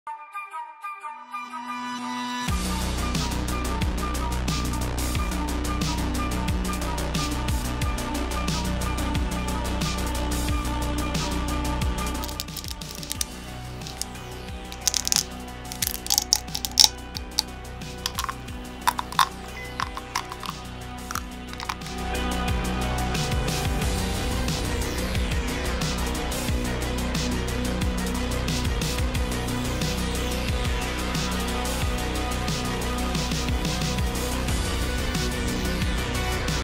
Mandalorian Cara Dune 3D Print
Mandalorian Cara Dune Resin 3D Printer Timelapse

Printed on the Elegoo Saturn ➜